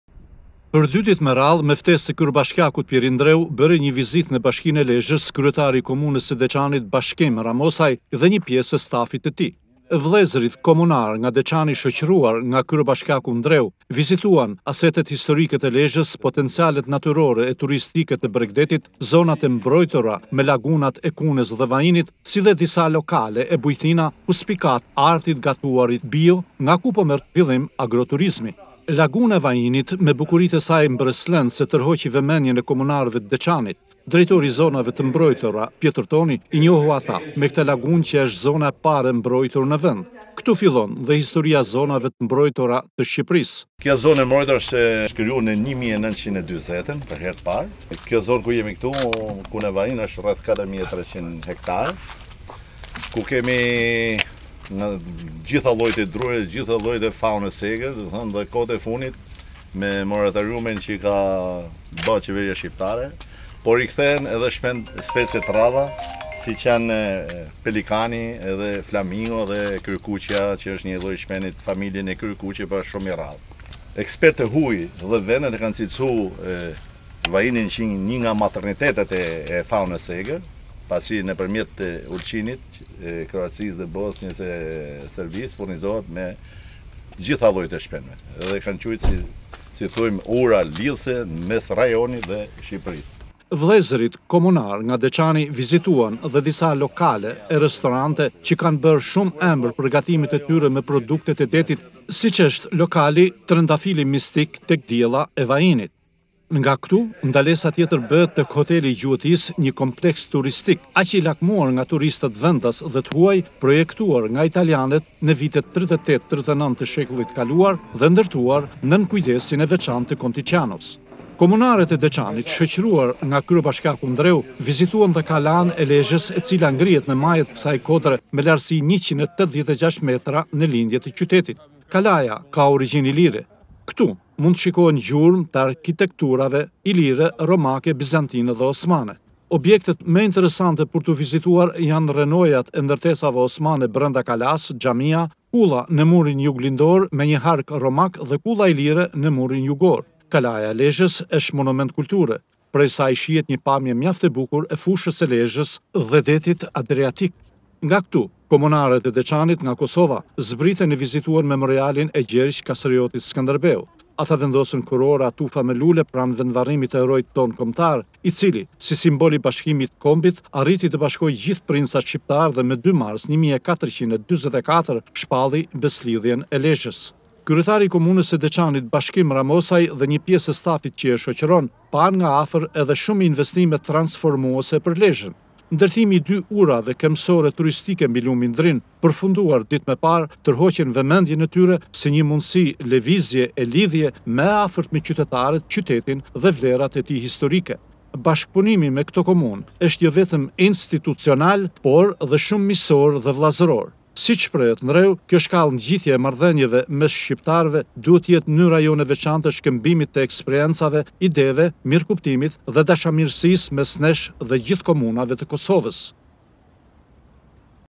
KOMUNA E DEQANIT VIZITON LEZHEN
Drejtues te Komunes se Deqanit kane zhvilluar nje takim  ne bashkine e Lezhes. Ata jane pritur nga kryebashkiaku Pjerin Ndreu, ndersa jane njohur nga afer edhe me resurset historike dhe turistike te Lezhes.